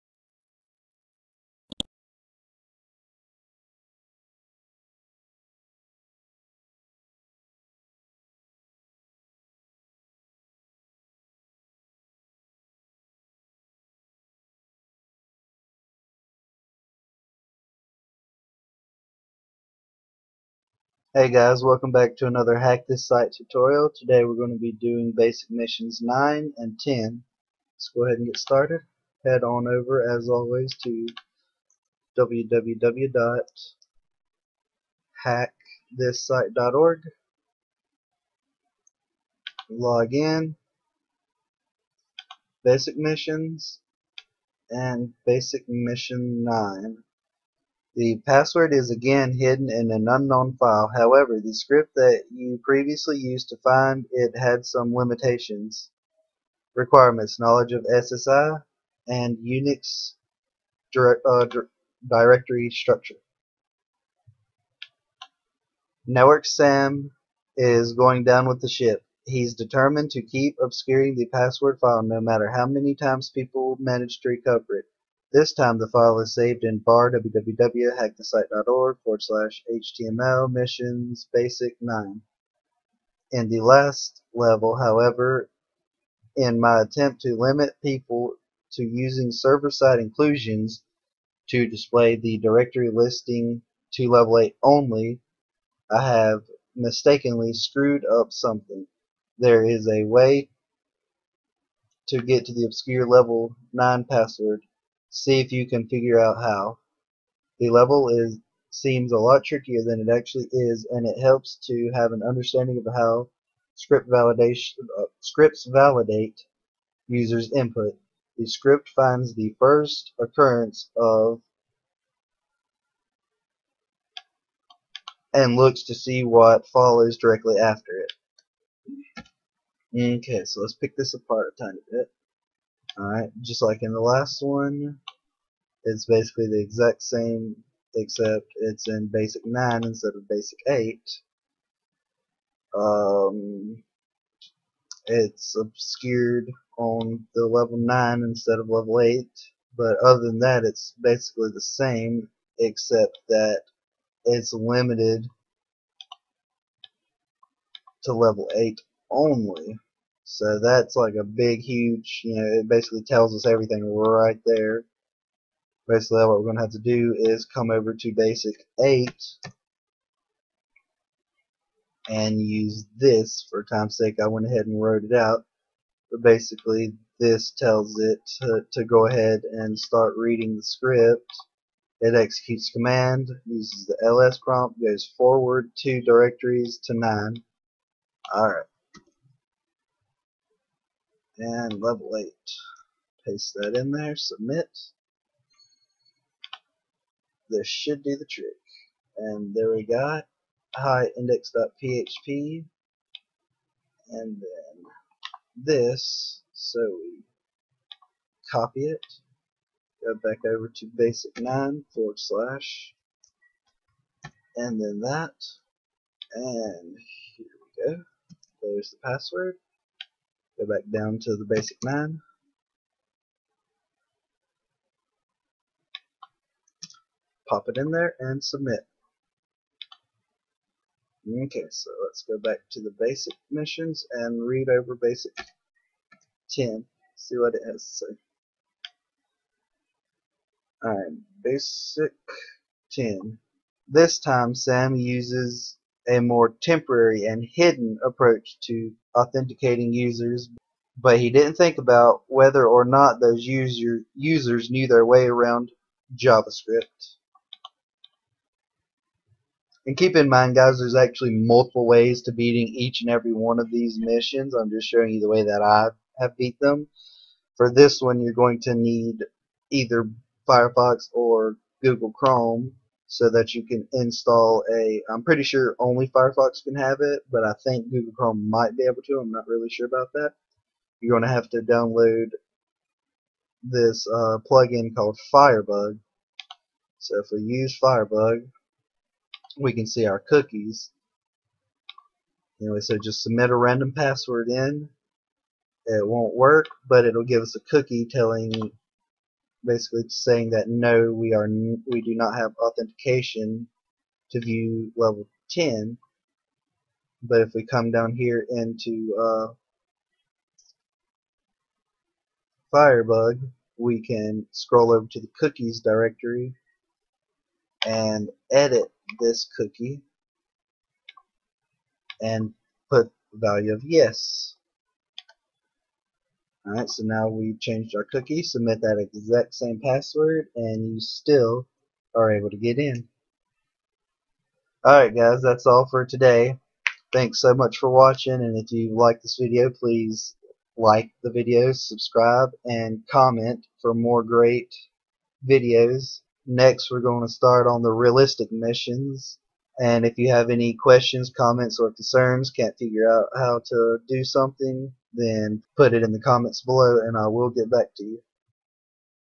Hack this site basic 9-10
fifth video in a series of hack this site tutorials.
Completing basic mission 9-10